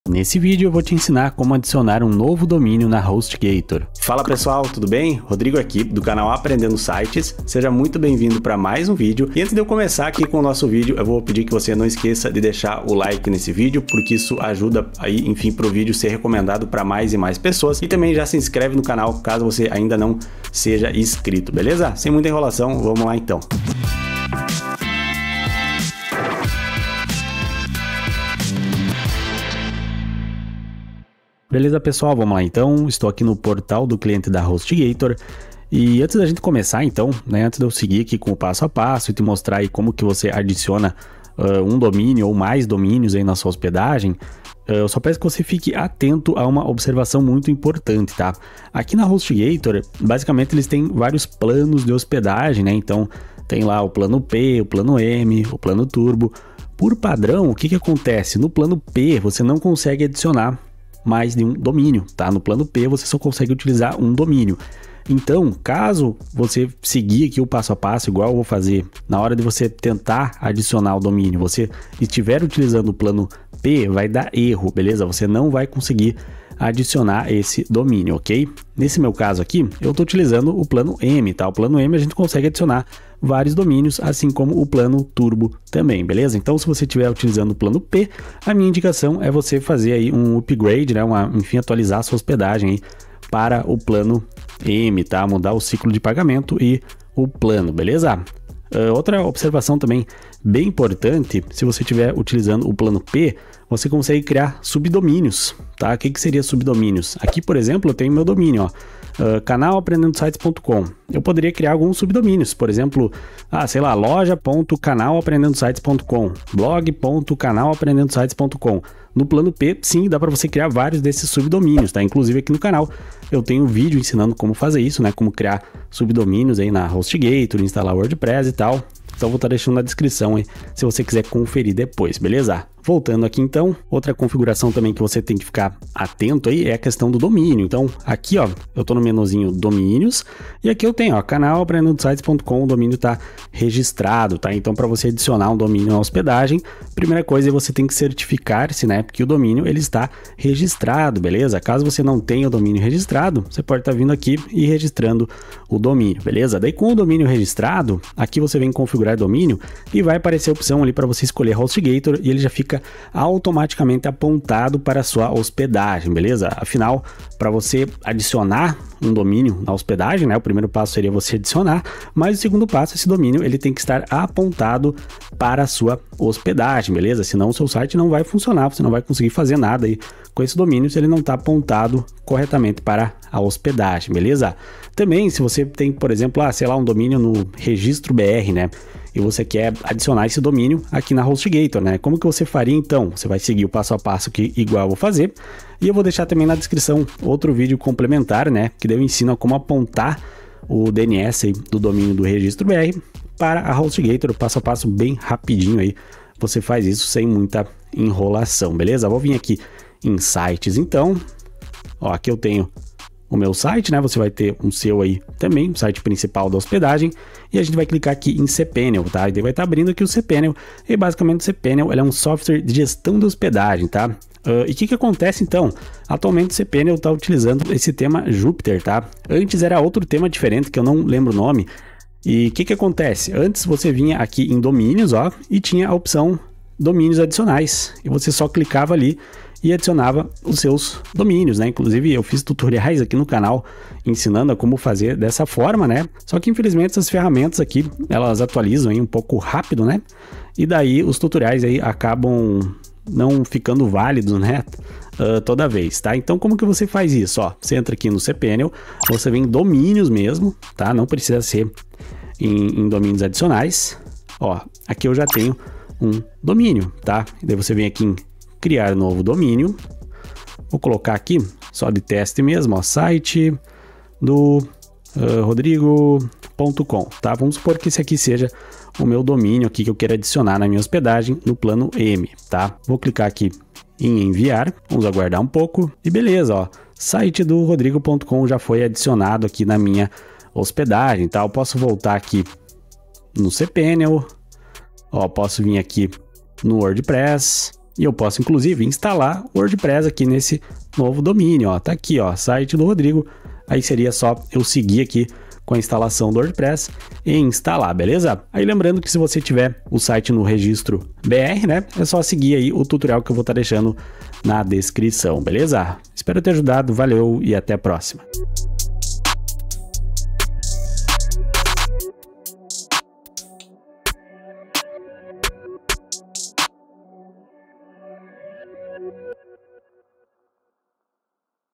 Como Adicionar NOVO DOMÍNIO na Hostgator | Atualizado 2025
Neste vídeo eu vou te ensinar como adicionar um novo domínio na Hostgator.